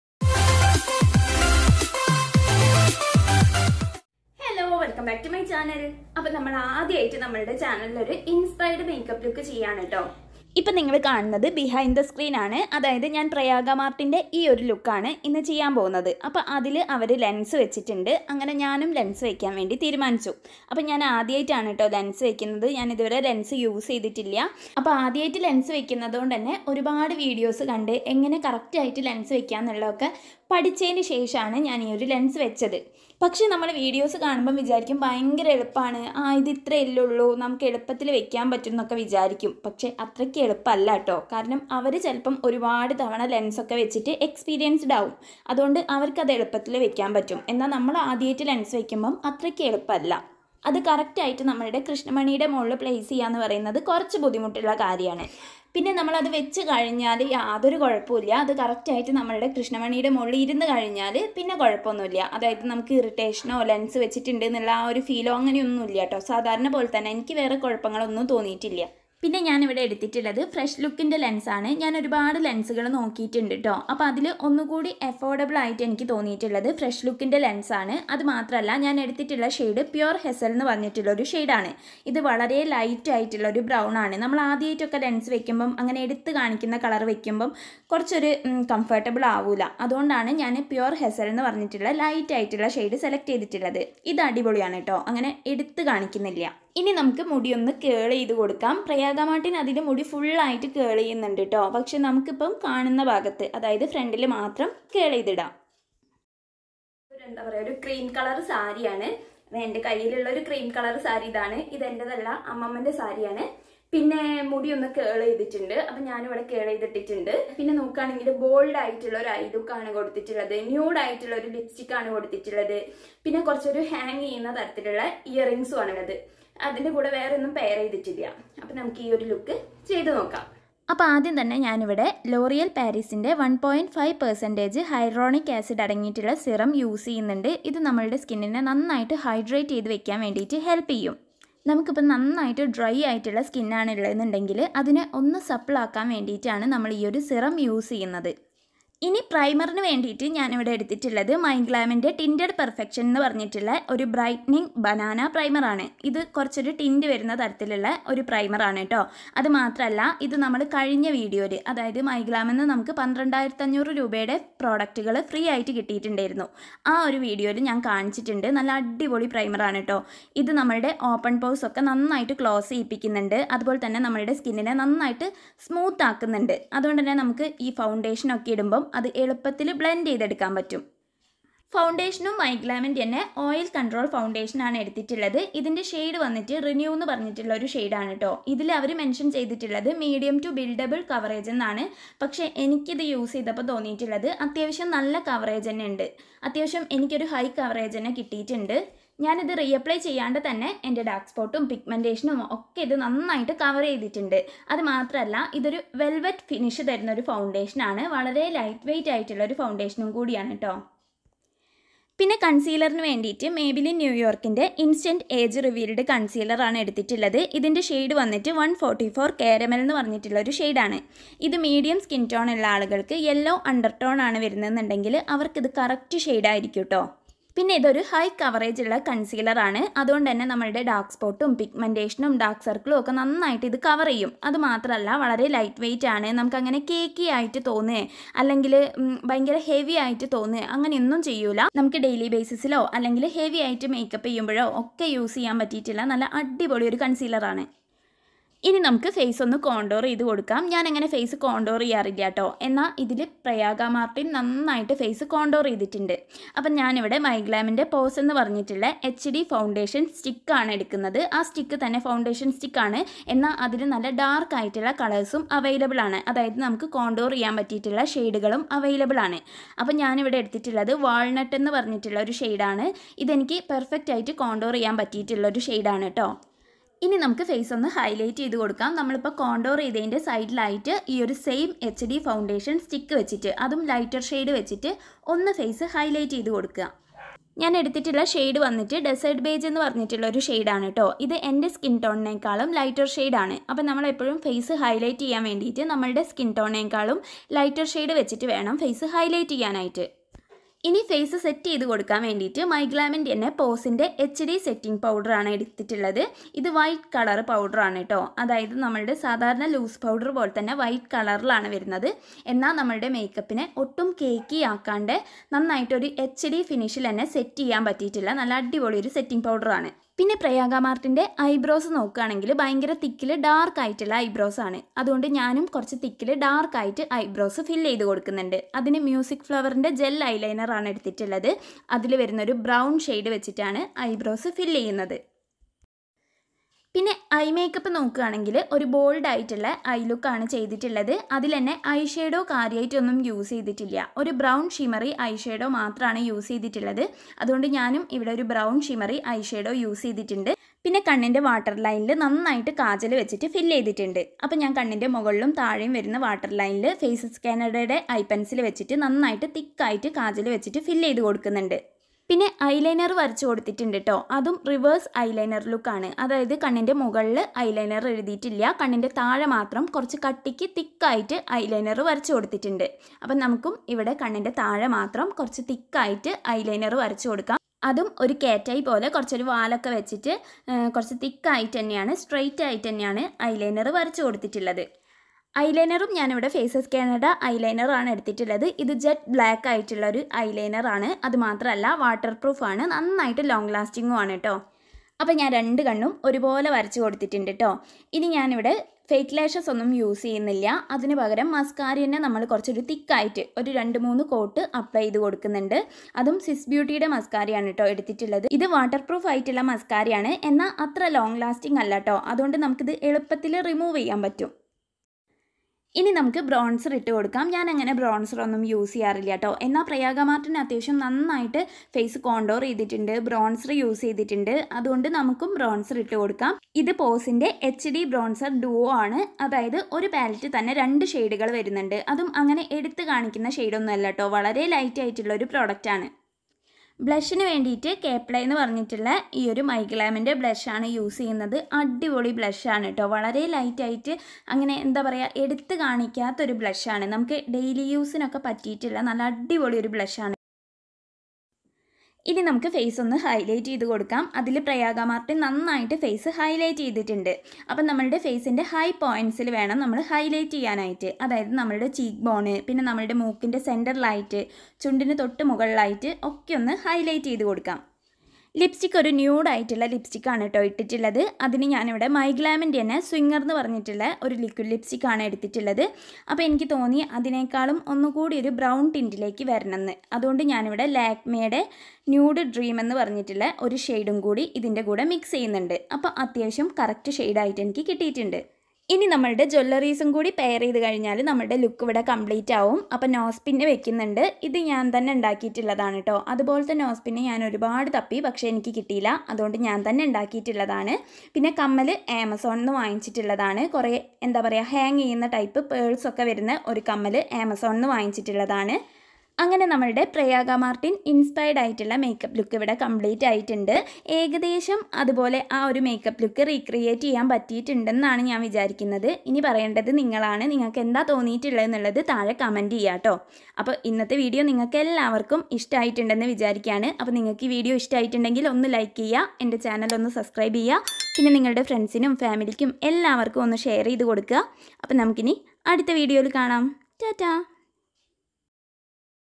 Prayaga Martin Inspired Makeup Look / Reverse Eyeliner Glam look / Trying Coloured Lens First Time🤦
12500/- രൂപയുടെ Makeup Products Free ആയിട്ടു കിട്ടിയാലോ !!! // Myglam Free makeup products Products